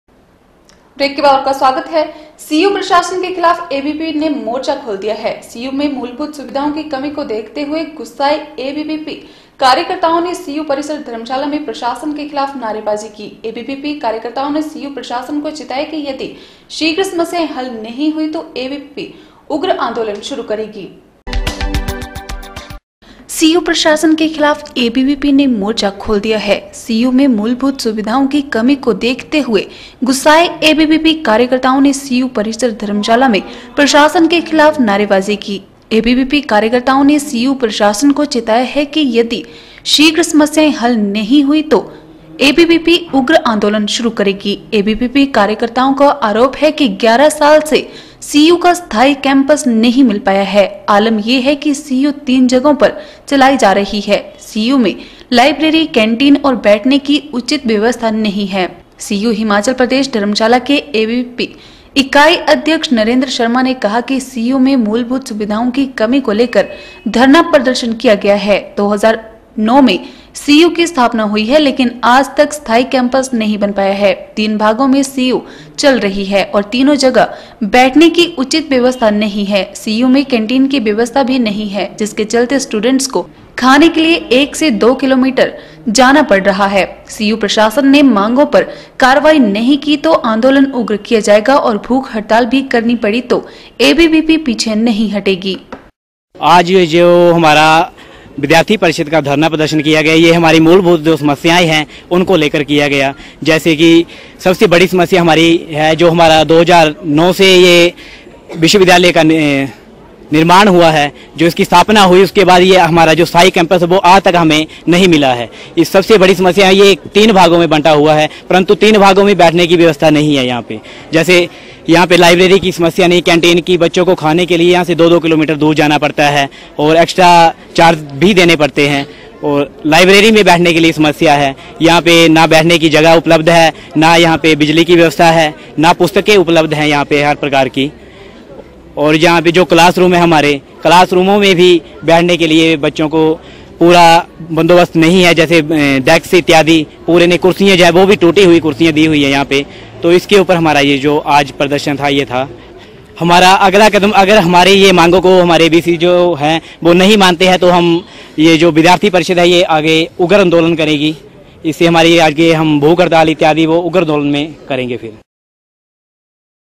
13 FEB N 8 सीयू प्रशासन के खिलाफ एबीवीपी ने मोर्चा खोल दिया
सीयू प्रशासन के खिलाफ एबीवीपी ने मोर्चा खोल दिया है। सीयू में मूलभूत सुविधाओं की कमी को देखते हुए गुस्साए एबीवीपी कार्यकर्ताओं ने सीयू परिसर धर्मशाला में प्रशासन के खिलाफ नारेबाजी की। एबीवीपी कार्यकर्ताओं ने सीयू प्रशासन को चेताया  है कि यदि शीघ्र समस्याएं हल नहीं हुई तो एबीवीपी उग्र आंदोलन शुरू करेगी।